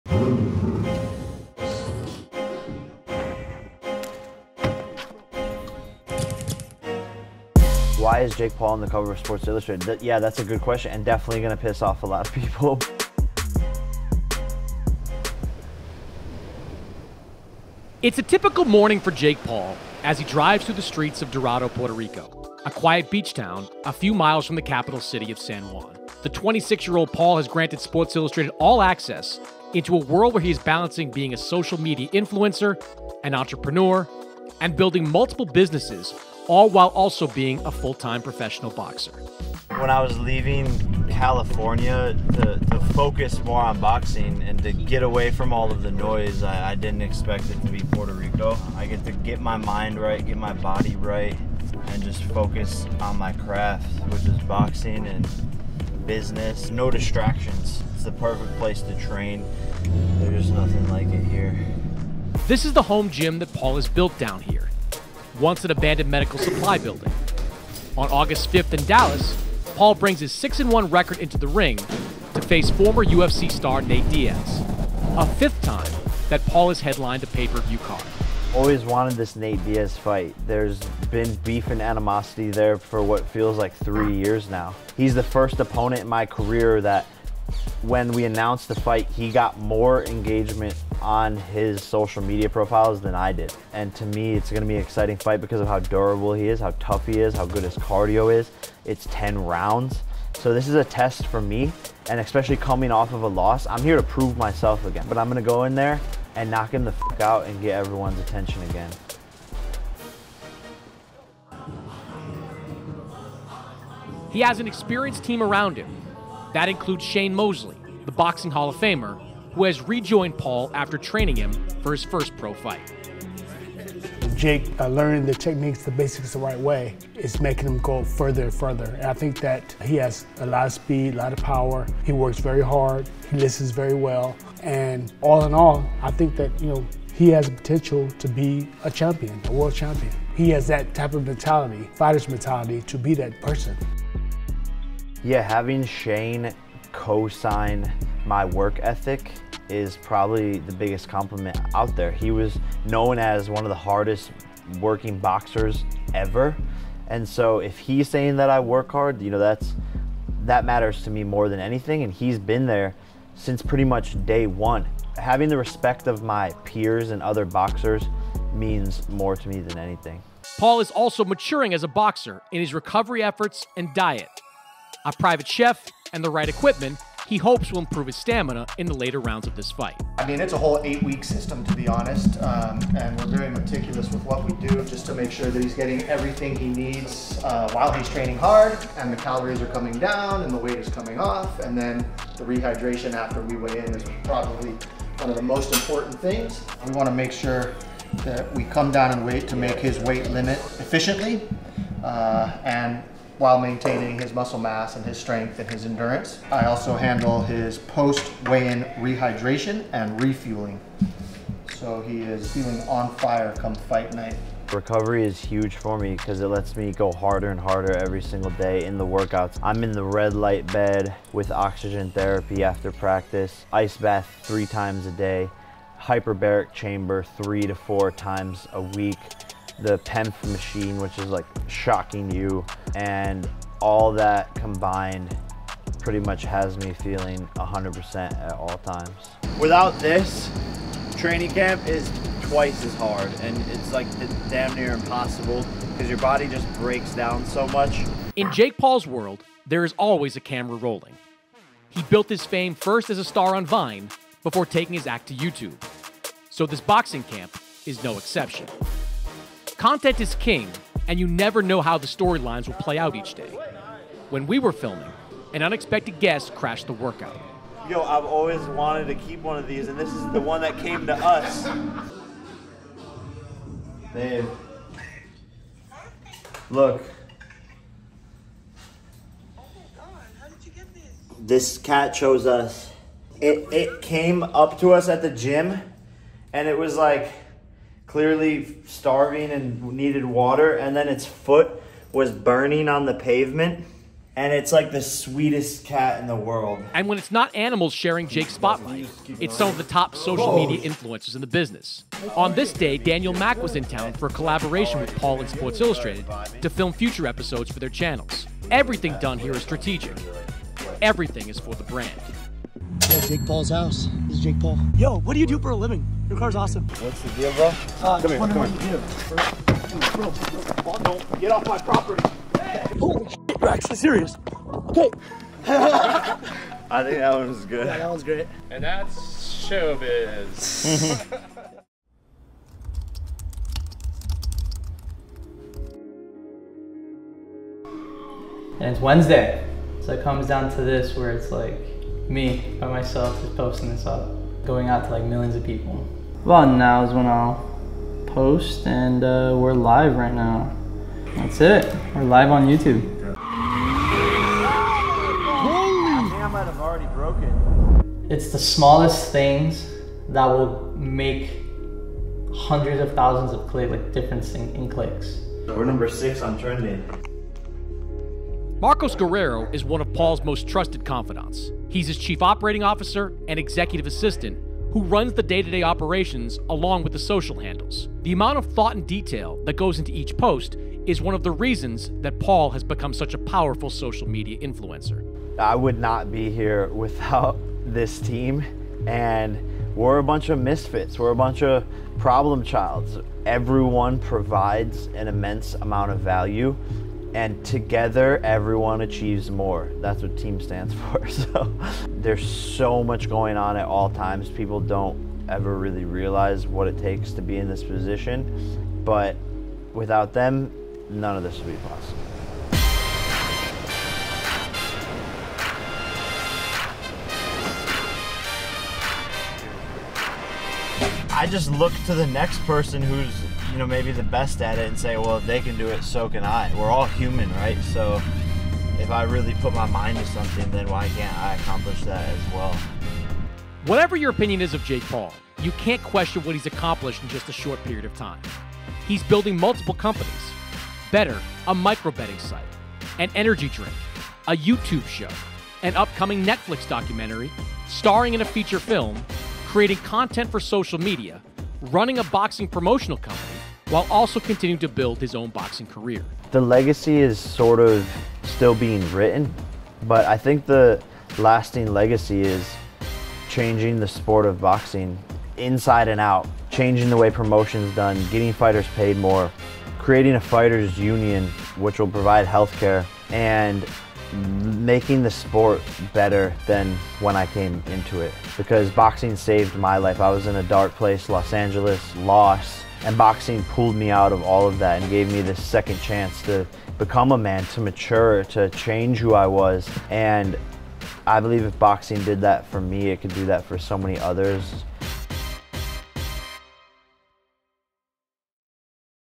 Being Jake Paul | Sports Illustrated
Boxer and social media influencer @jakepaul takes SI behind the curtain and into his world, living, working, and training in Puerto Rico as he prepares for his highly anticipated fight against Nate Diaz.

A special thank you to @itsdanielmac for coming out to collab with Jake for SI!